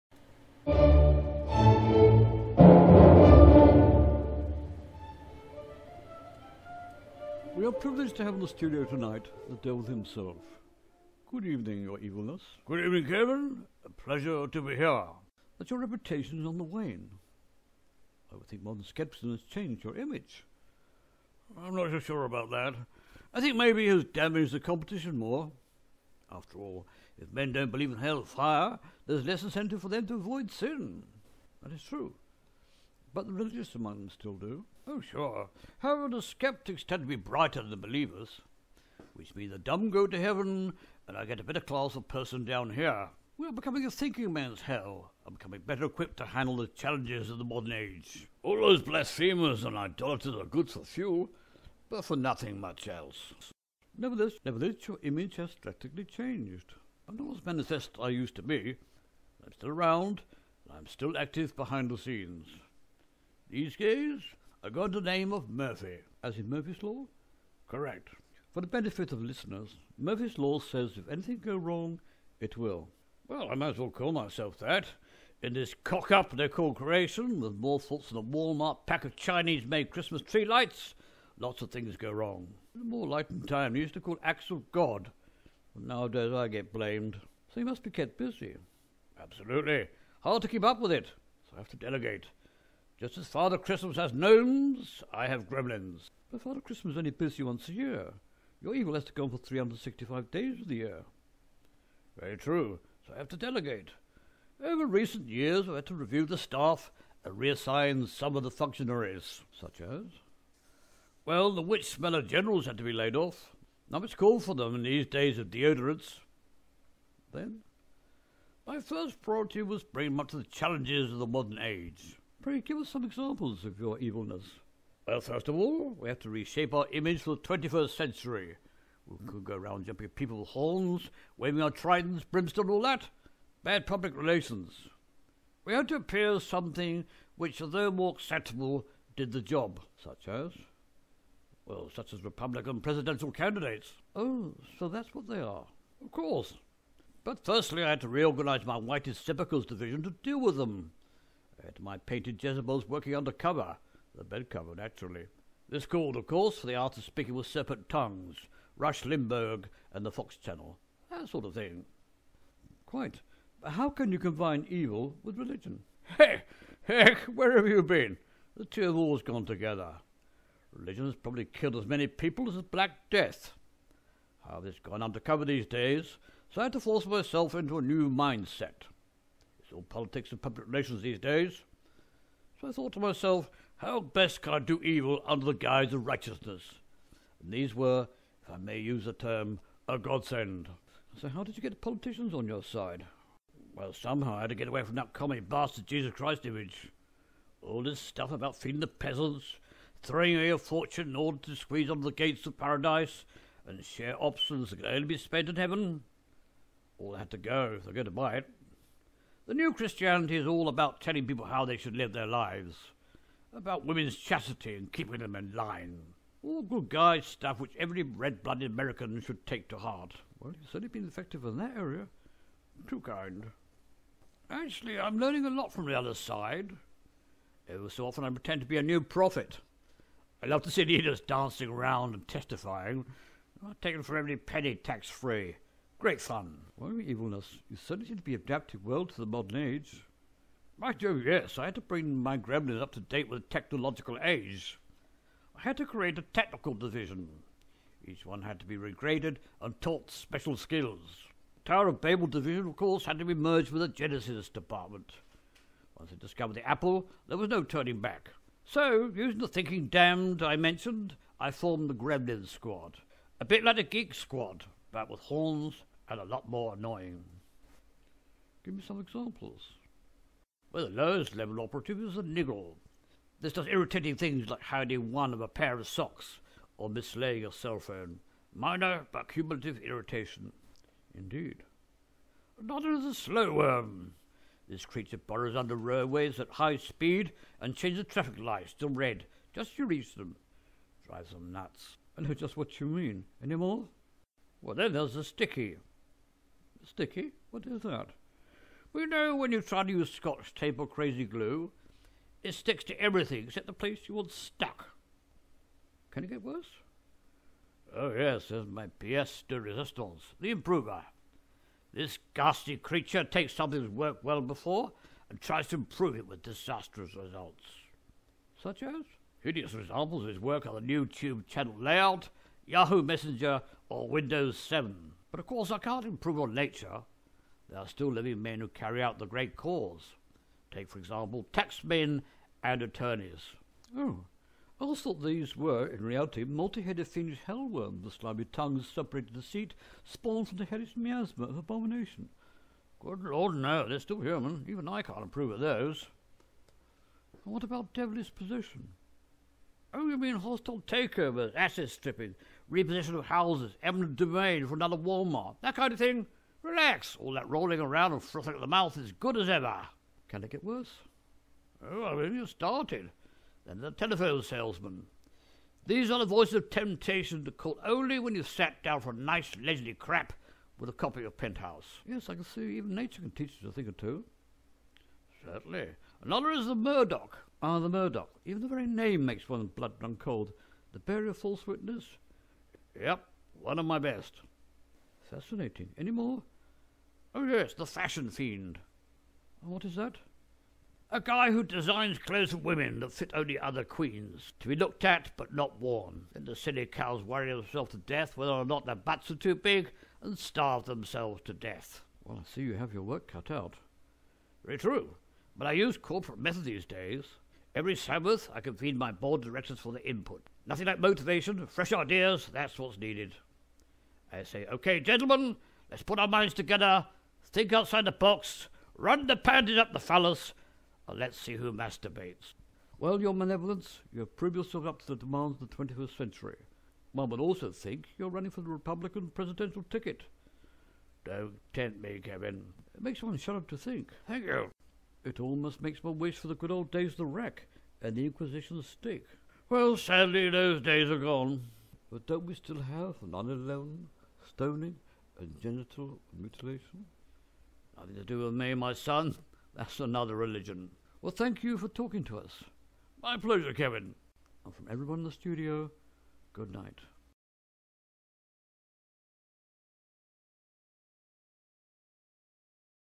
Interview with the Devil on Fox News.
The only place the Devil will permit himself to appear on is Fox News.  Here is the interview.